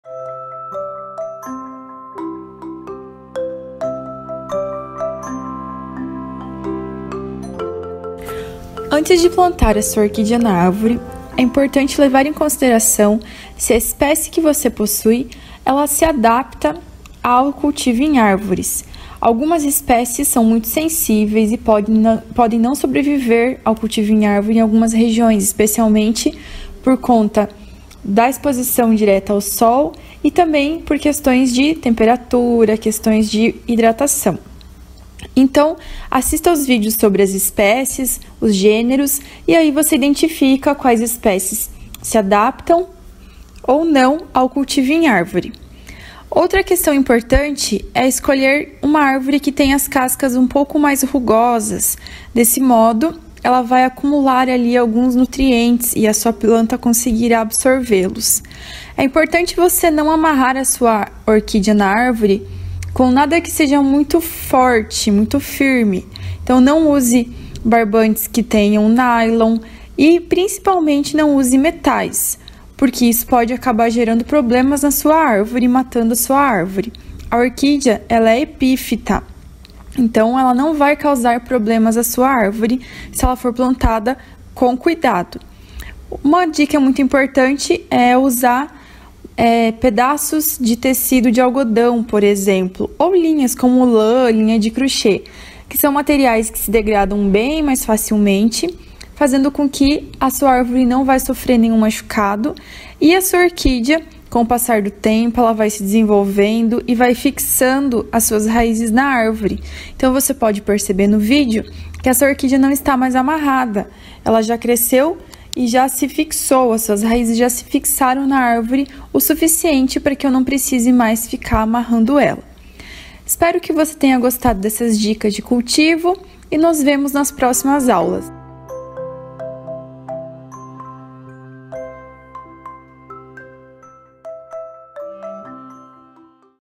Como plantar orquídeas em árvores?
Neste vídeo apresentamos como cultivar orquídeas em arvores, explicando sobre como cuidar melhor e desmitificando alguns mitos sobre o cultivo delas em arvores.

Este vídeo é um dos resultados do projeto “Eu Cultivo Orquídeas” desenvolvido na UTFPR.
Este vídeo faz parte da playlist: Cultivo de orquídeas para iniciantes.

Este vídeo foi gravado em 2020, no Garden Matelândia.
Imagens: Antonio Guilherme Basso Pereira e Betty Cristiane Kuhn
Edição: Guilherme Lago